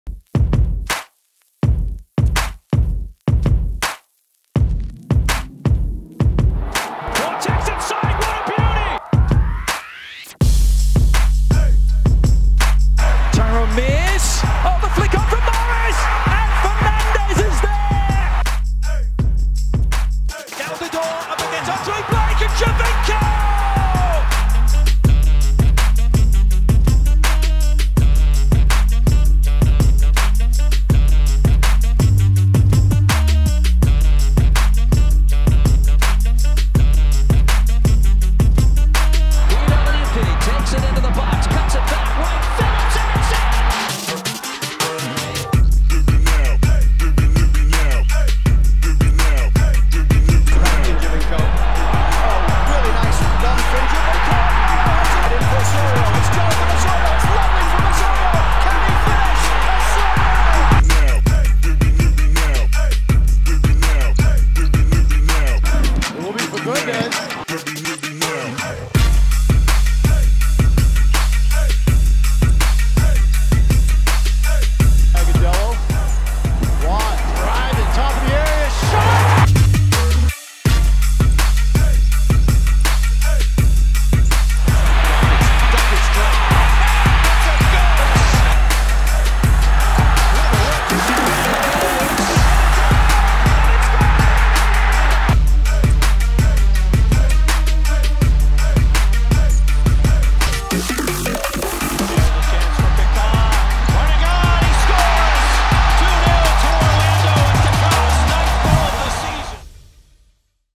Giovinco was a savage in October
Check out the best flicks, nutmegs, tricks, and skill plays from the month of October.